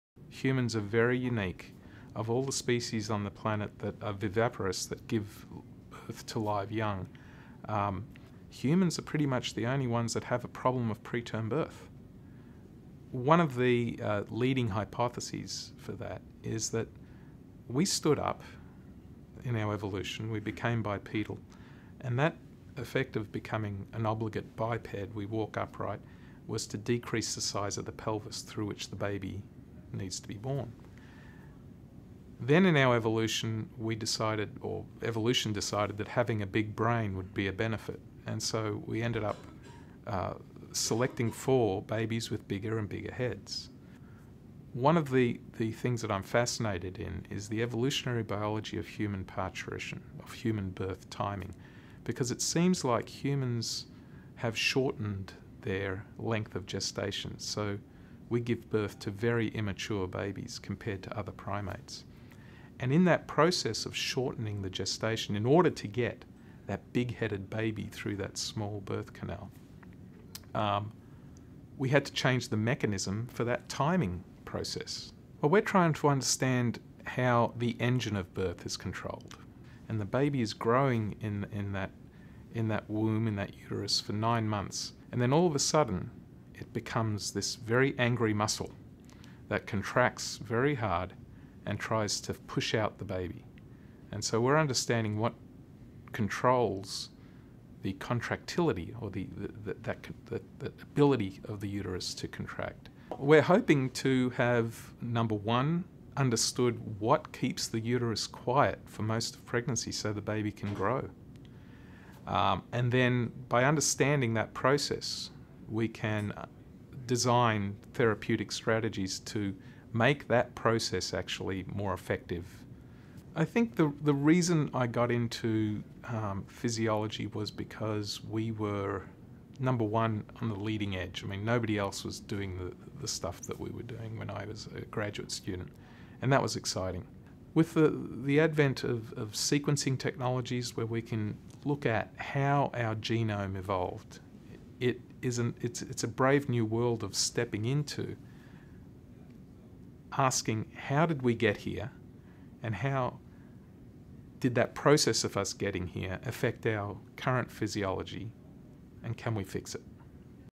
Dr. Sam Mesiano - Preventing Preterm Birth initiative
Five innovative research projects aiming to prevent premature birth have been announced by the Global Alliance to Prevent Prematurity and Stillbirth (GAPPS), an initiative of Seattle Children's.

The projects are funded through the Preventing Preterm Birth initiative, part of the Bill & Melinda Gates Foundation's Grand Challenges in Global Health. The initiative seeks to discover biological mechanisms that lead to preterm birth and develop innovative strategies for prevention, with particular focus on solutions relevant to low- and middle-income countries, where 99% of the world's infant deaths occur.